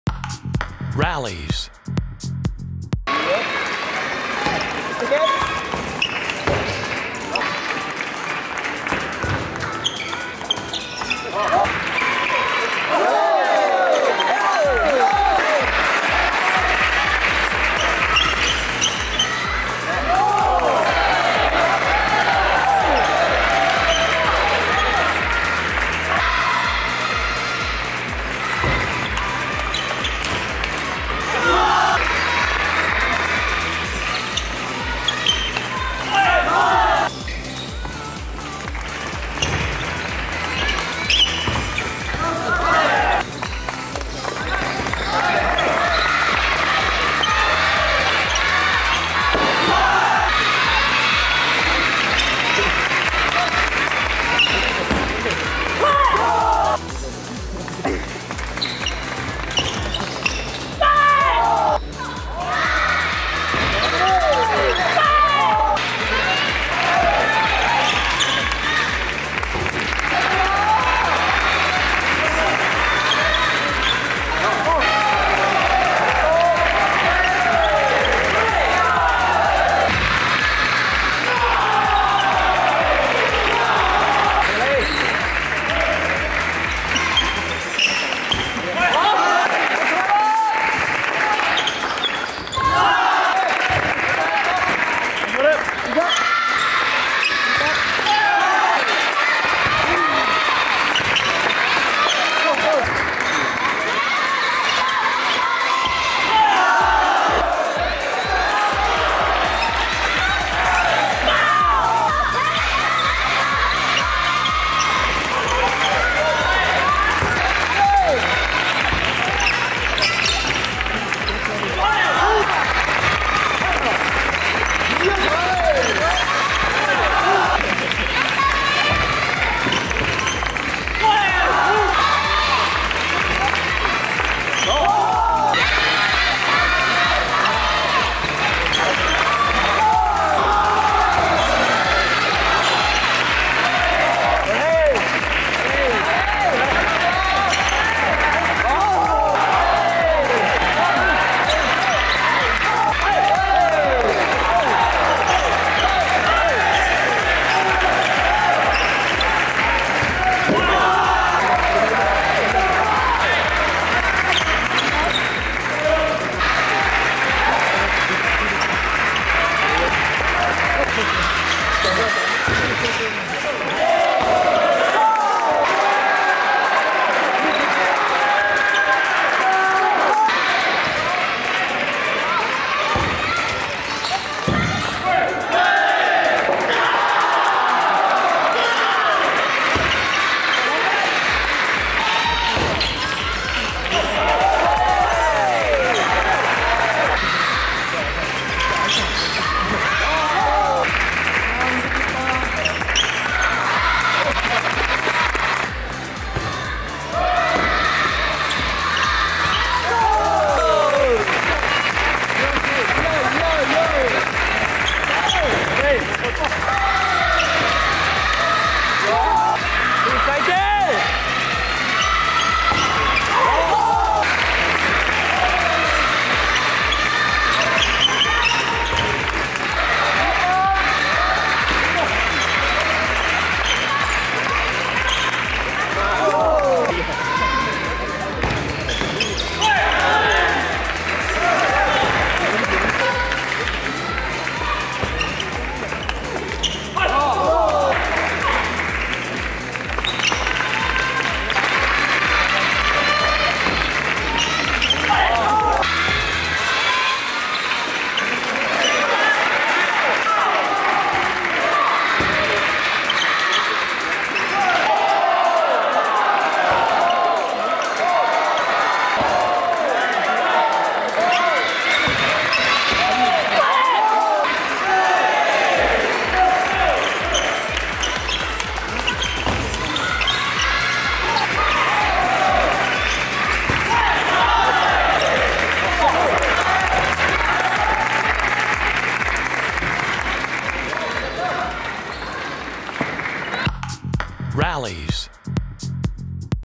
森薗政崇(明治大) vs大塚大寛(日本大)H29春 関東学生卓球リーグ | Rallys（ラリーズ）
【本動画の試合解説はこちら】

【Time Table】
0:00-1セット目
1:58-2セット目
3:26-3セット目

【試合情報】
大会名：平成29年度春季 関東学生卓球リーグ戦 第3戦 男子 明治大 対 日本大
選手名：森薗政崇（明治大） 対 大塚大寛（日本大）
大会種別：団体戦、シングルス、国内、関東学生リーグ
試合結果：森薗3-0大塚
フルバージョン動画：